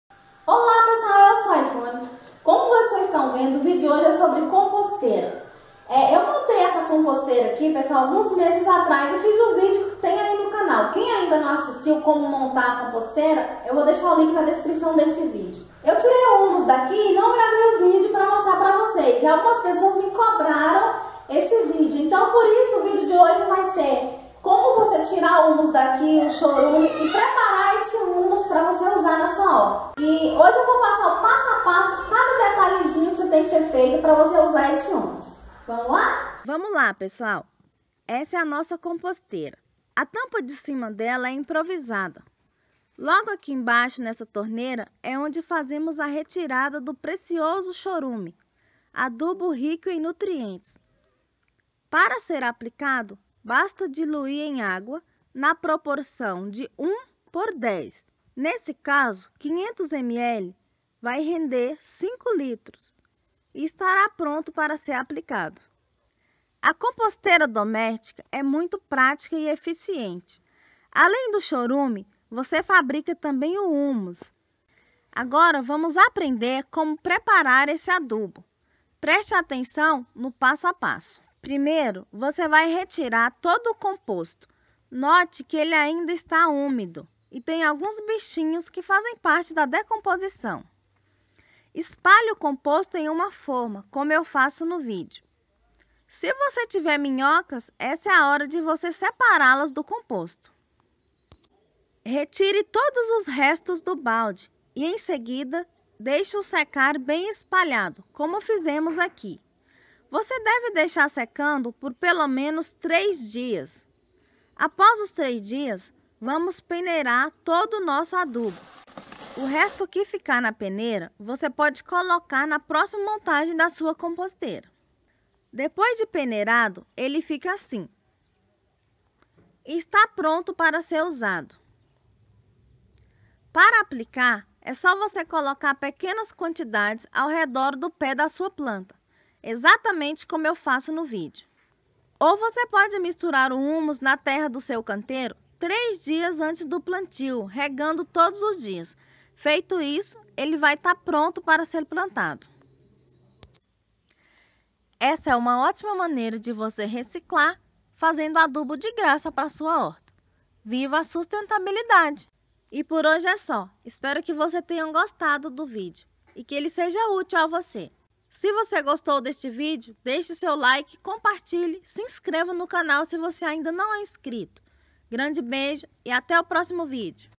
Humus da Composteira, passo a passo de como retirar e utilizar
PARTICIPE DA NOSSA VAQUINHA Clique no link e conheça nosso projeto. Sua colaboração será muito bem vinda.Neste vide aprenda a retirar o humus da composteira e como utilizar nas suas plantas.
Composteira com 2 baldes monte a sua passo a passo;